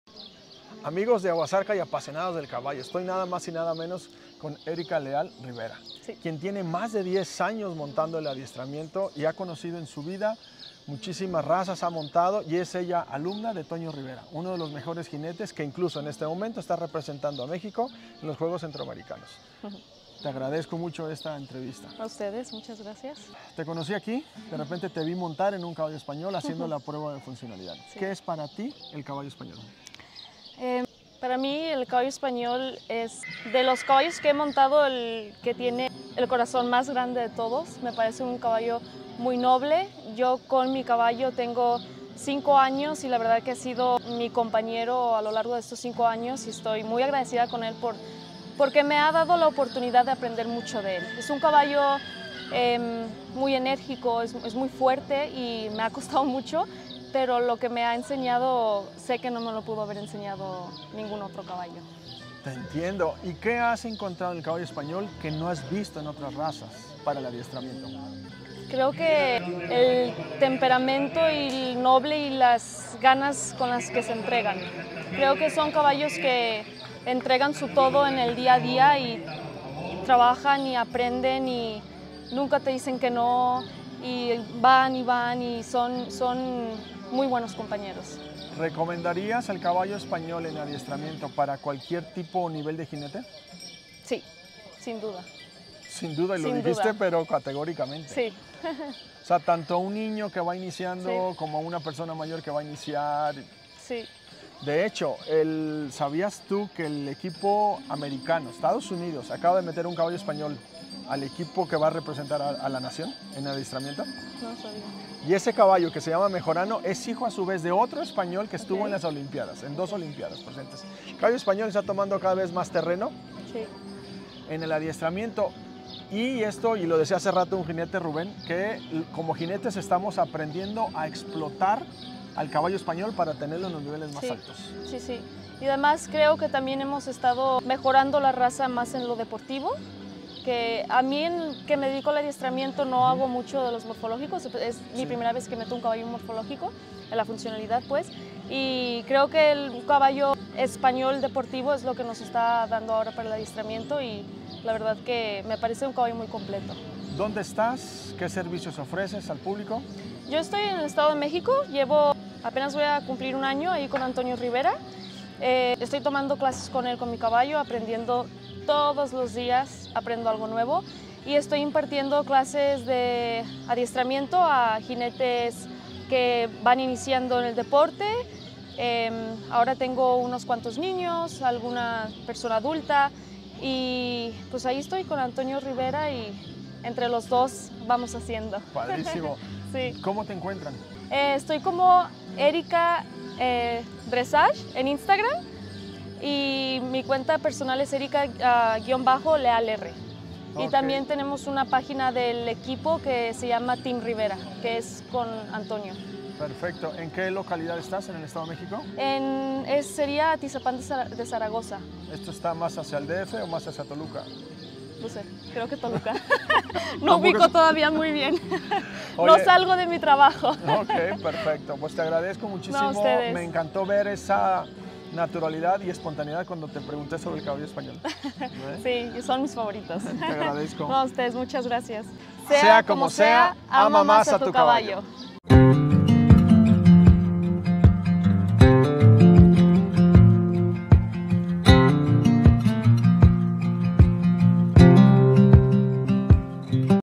Una Mirada al Caballo Español para el Adiestramiento/Doma Clásica...
Aguas!!! Como Jinetes y Ganaderos estamos iniciando a Explotar las habilidades del Caballo Español en el Adiestramiento/Doma Clásica  …

Aguas !!!
El Caballo Español llenará las pistas e hípicos de Adiestramiento.

Quieres conocer los Caballos que tenemos en Venta, manda la palabra CABALLO por Privado y nos ponemos en contacto contigo

Agua Zarca,
Sea como Sea,
Ama + a Tu Caballo